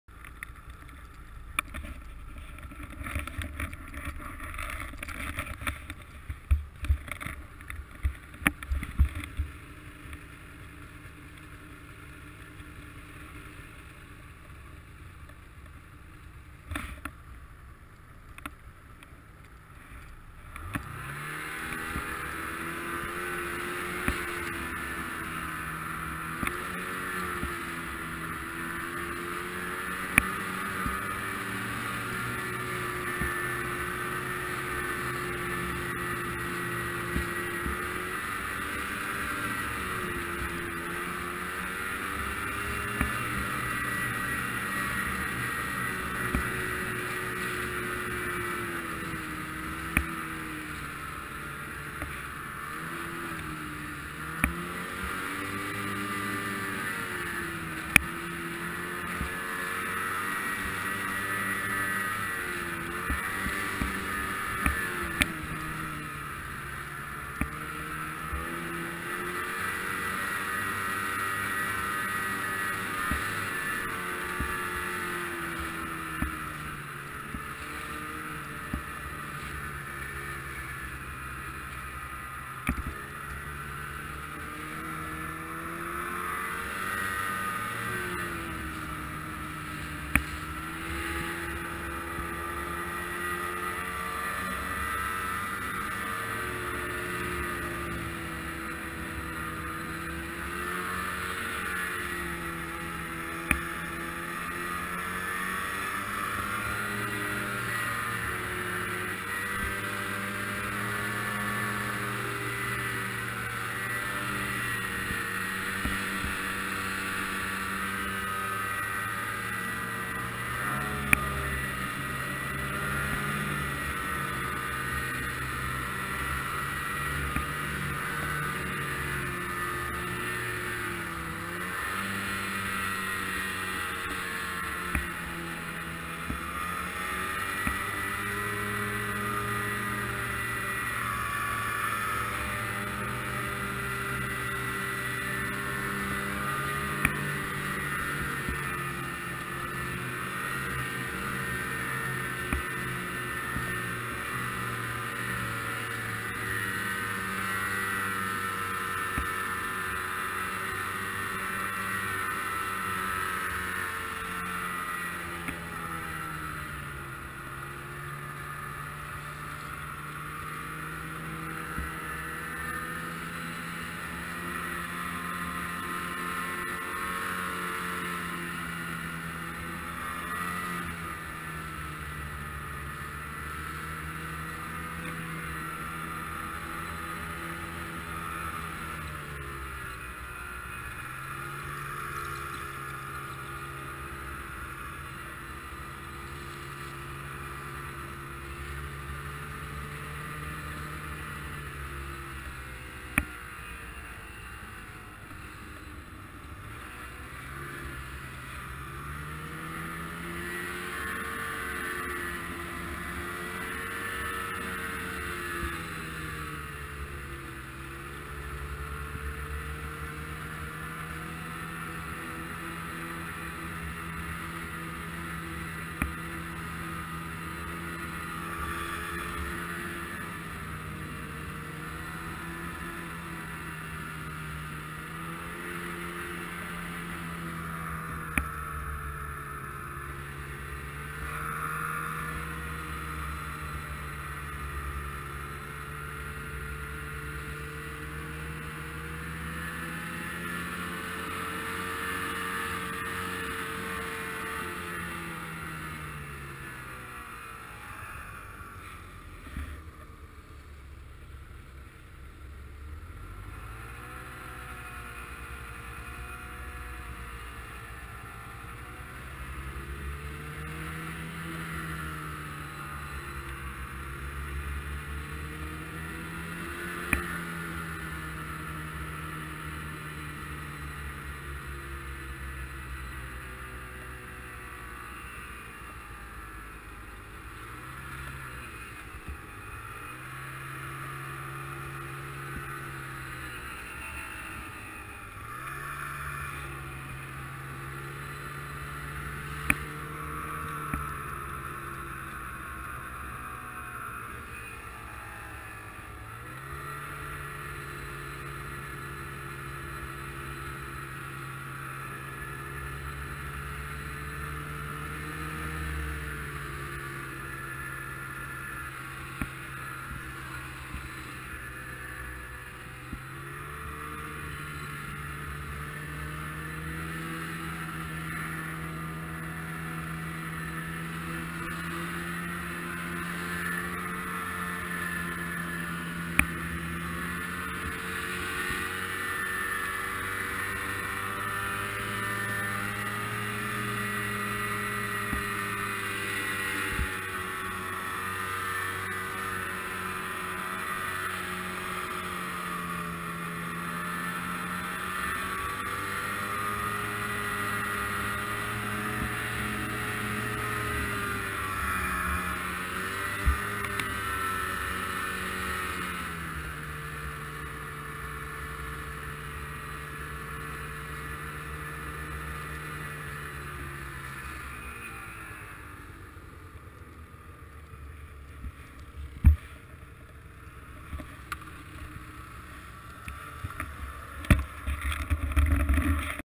heading north on grizzly ridge down into the burn
Riding grizzly ridge with Wade D mar 2017